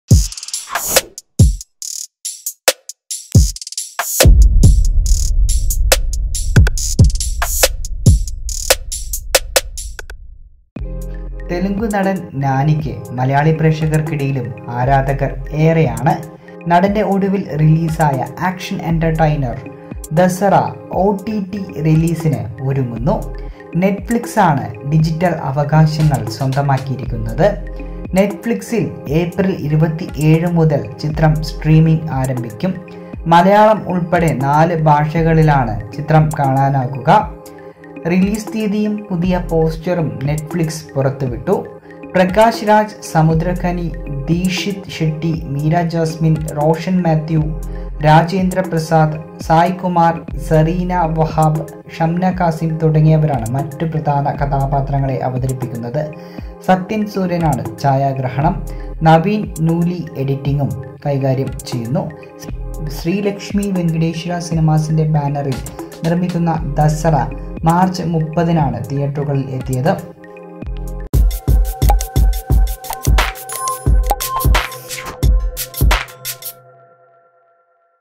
'ദസറ'ഒ.ടി.ടി റിലീസിന്, നാലു ഭാഷകളിൽ കാണാം
തെലുങ്ക് നടൻ നാനിക്ക് മലയാളി പ്രേക്ഷകർക്കിടയിലും ആരാധകർ ഏറെയാണ്. നടൻറെ ആക്ഷൻ എന്റർടൈനർ 'ദസറ'ഒ.ടി.ടി റിലീസിന് ഒരുങ്ങുന്നു.

നെറ്റ്ഫ്ലിക്സിൽ ഏപ്രിൽ 27 മുതൽ സ്ട്രീമിംഗ് ആരംഭിക്കും. മലയാളം ഉൾപ്പെടെ നാല് ഭാഷകളിൽ ചിത്രം കാണാനാകും.